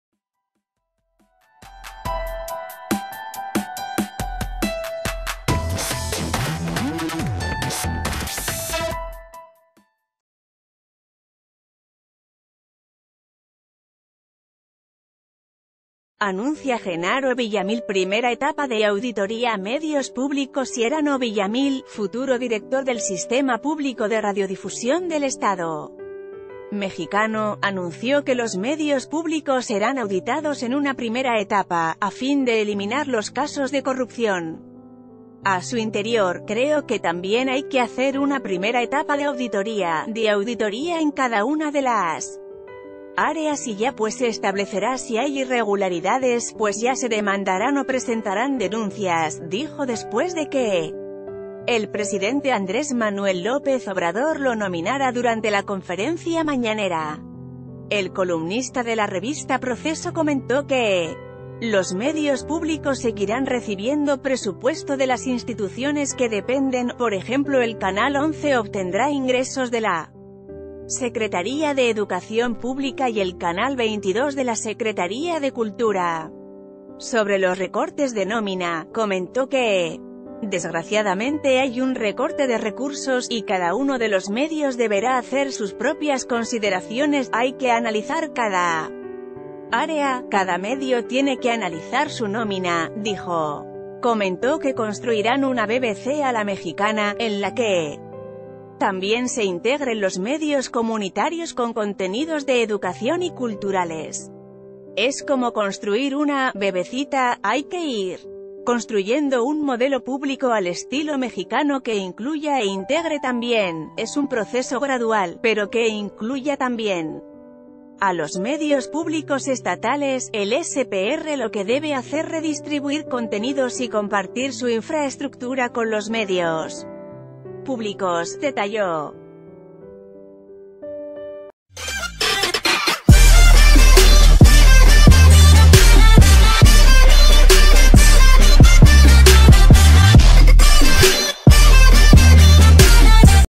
Anuncia Jenaro Villamil primera etapa de auditoría a medios públicos
Anuncia Jenaro Villamil primera etapa de auditoría a medios públicosJenaro Villamil, futuro director del Sistema Público de Radiodifusión del Estado Mexicano, anunció que los medios públicos serán auditados en una primera etapa, a fin de eliminar los casos de corrupción a su interior.“Creo que tambi...